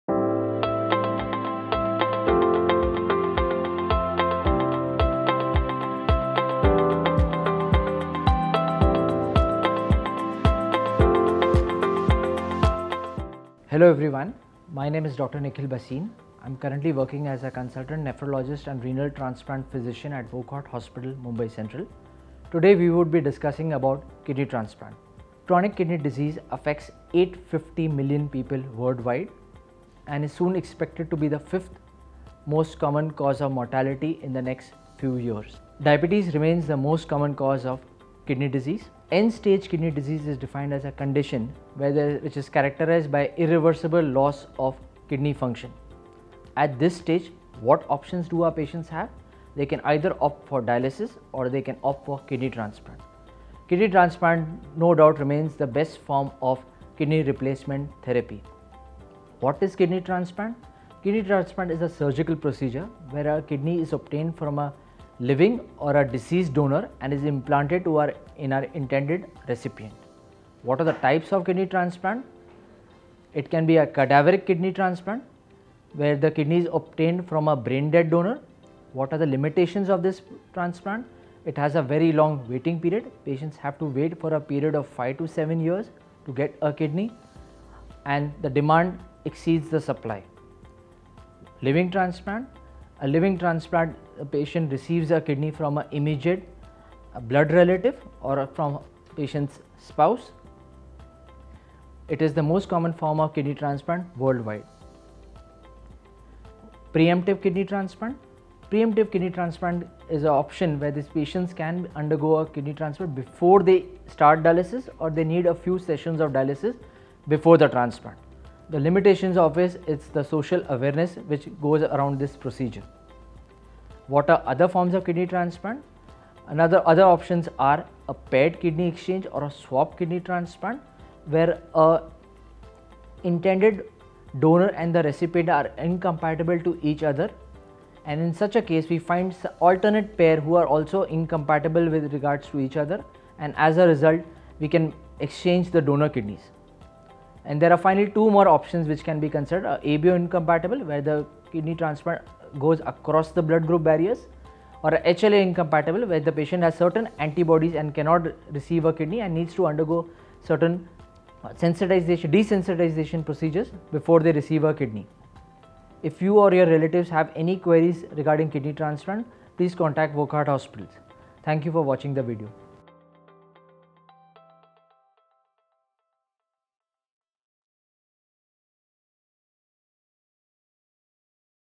Kidney Transplant - Dr Nikhil Bhasin
In the realm of medical marvels, kidney transplants stand out as lifesaving interventions. This procedure involves replacing a failed kidney with a healthy one, either from a living or deceased donor. Whether from a close relative or a generous stranger, a successful transplant can dramatically improve the recipient's quality of life. From renewed energy to reduced dependence on dialysis, kidney transplants offer a second chance at health and vitality.

Explore the effective treatments with our renowned Consultant Nephrologist Dr Nikhil Bhasin, Wockhardt Hospitals, Mumbai Central.